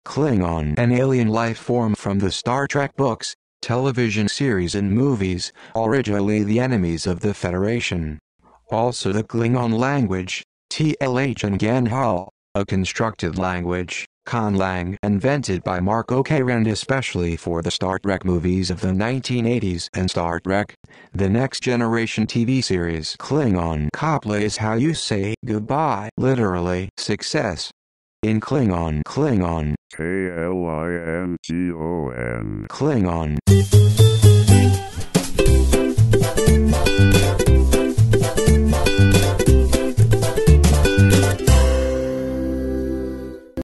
klingon meaning and pronunciation
An alien life form from the Star Trek books, television series and movies, origially the enemies of the Federation. Also the Klingon language, tlhIngan Hol, a constructed language (conlang) invented by Mark Okrand especially for the Star Trek movies of the 1980s and Star Trek: The Next Generation TV series.

klingon definition by Urban Dictionary